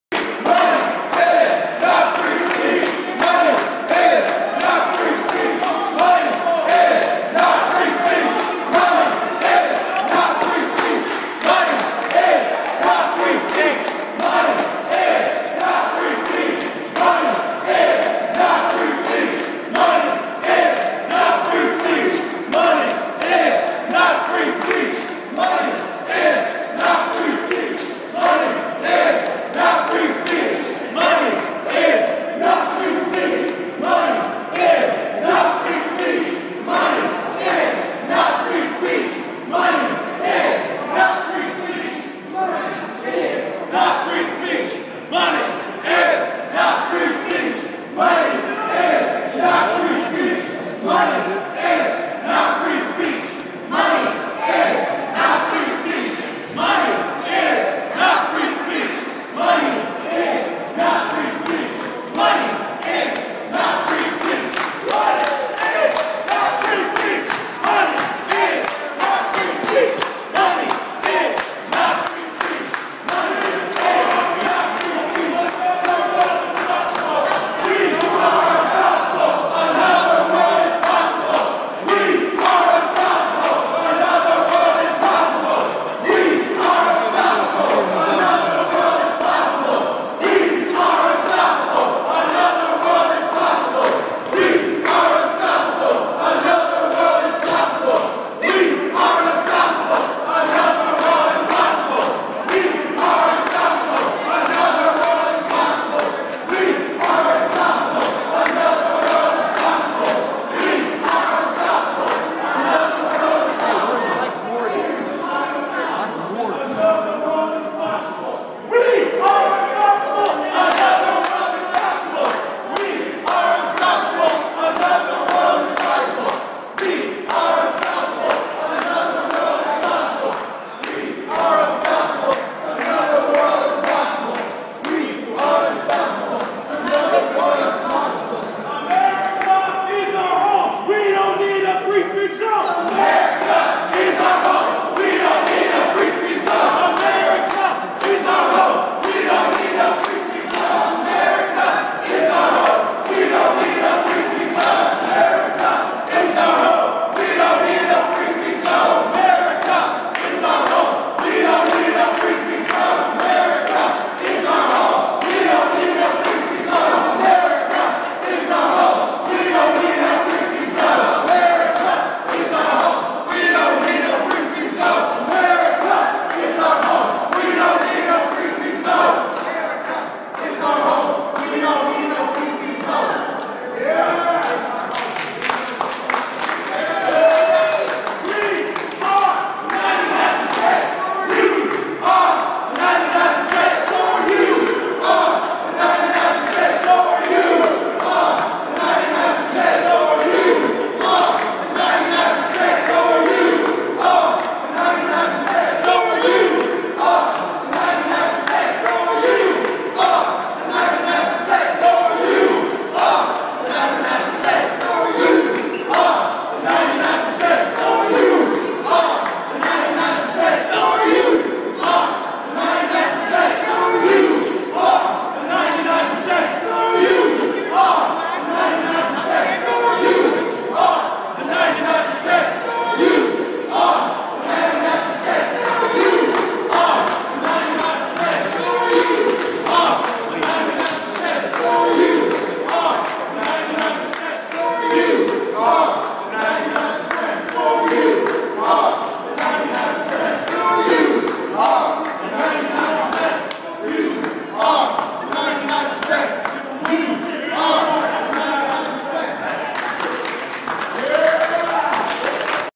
OccupyCLT
DNC2012 ordinance has passed. The reaction to Charlotte city council at the government bldg.